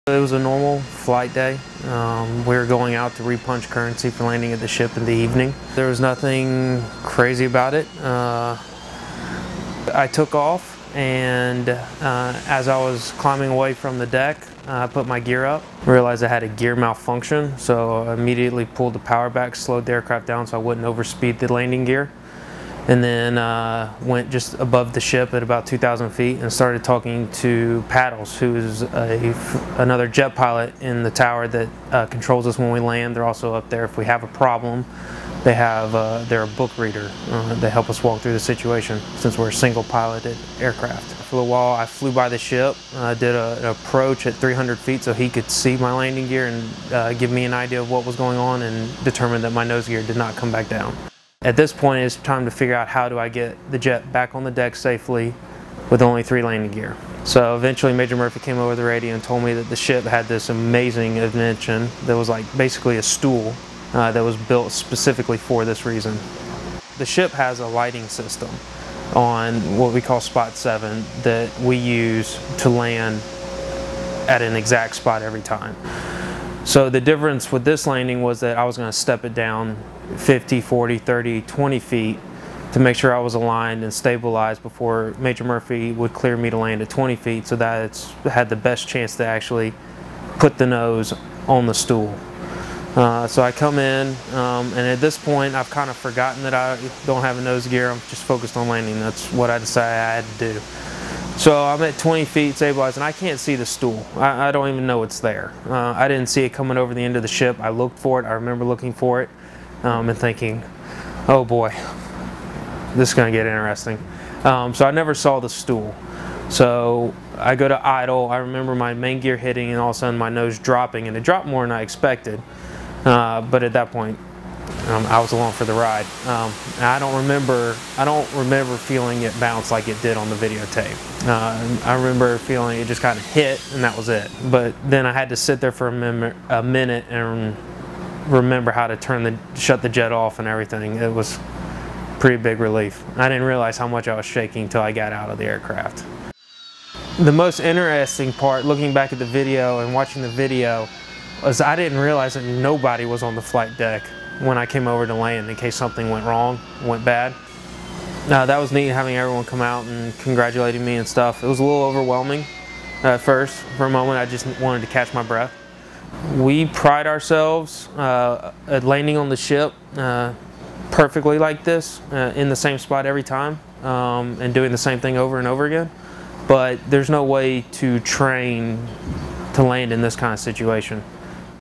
AV-8B Harrier Emergency Carrier Landing | Front Gear Failure
U.S. Marine Corps Capt. William Mahoney, Marine Medium Tiltrotor Squadron (VMM) 263 (Reinforced), 22nd Marine Expeditionary Unit (MEU), AV-8B Harrier jet pilot, talks about his experience during a controlled landing after his front landing gear malfunctioned on his aircraft aboard the USS Bataan (LHD 5) at sea, June 7, 2014. Elements of the MEU, embarked aboard the Bataan, are operating in the U.S. 6th fleet area of operations to augment U.S. Crisis Response forces in the region. Video by Sgt. Alisa Helin.